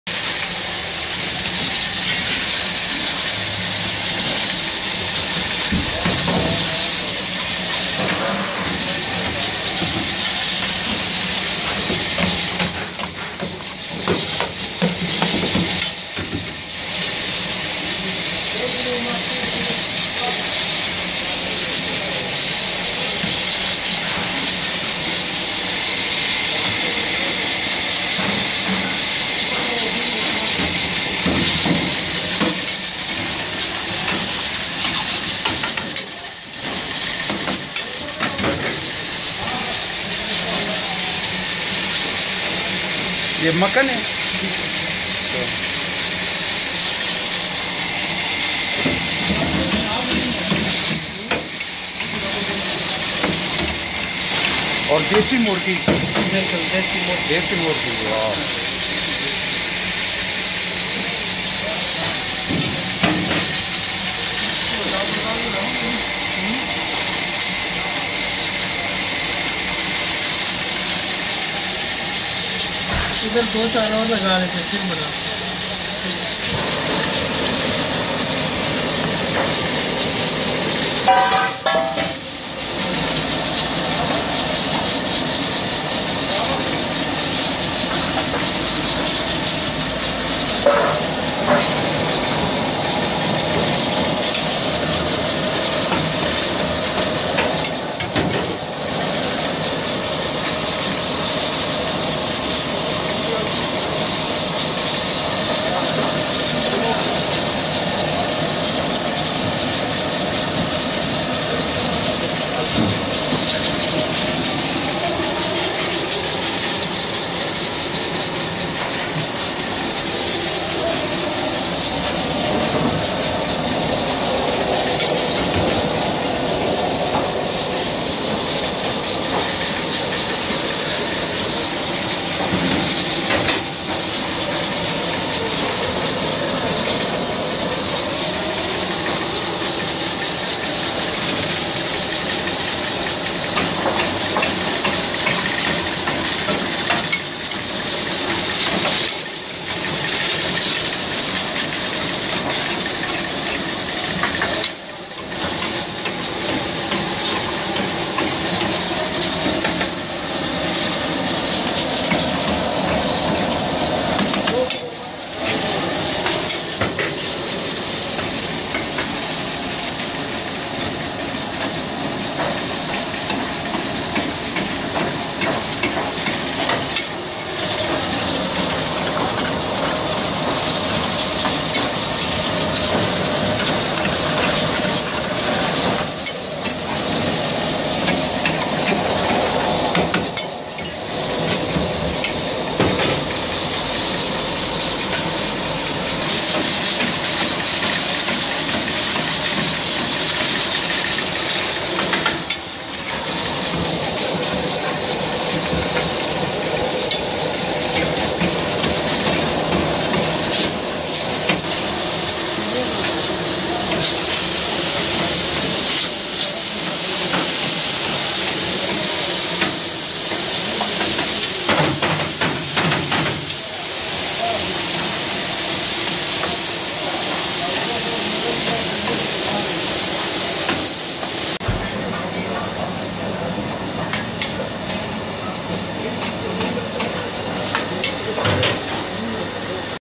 Lahori Butt Karahi in Peshawar | Butt Chicken Karahi | Desi Murgh Karahi
Location : Hujra Restaurant - Ring Road Peshawar